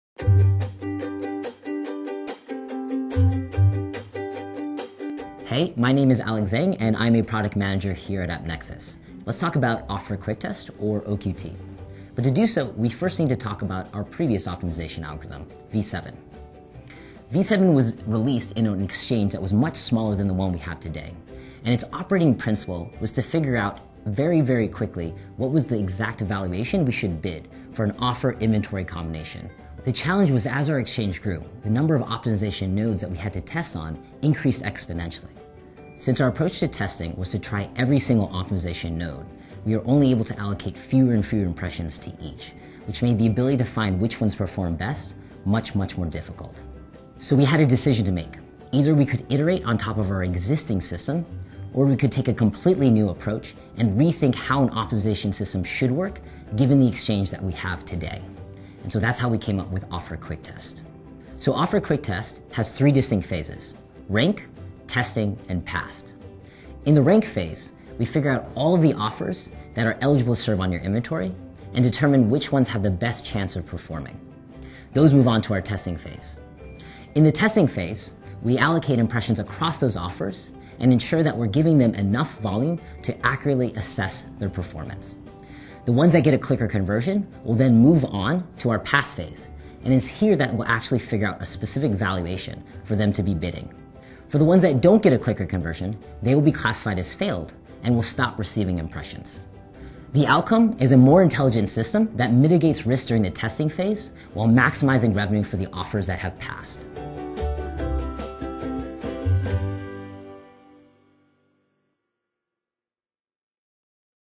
Meet Our New Optimization Engine OQT
Learn how AppNexus completely re-eningeered our optimization engine to provide clients with increased campaign performance